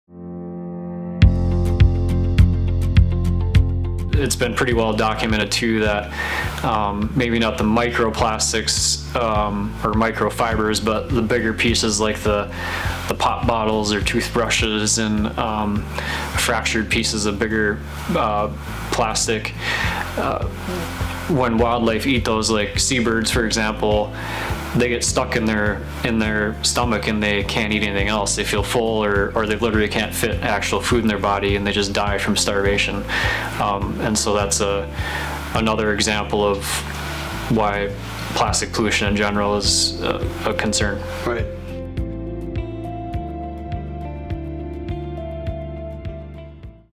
The Environmental Impact on Wildlife
How does plastic affect the wellbeing of nature? Here's how our pollution is endangering the lives of wildlife.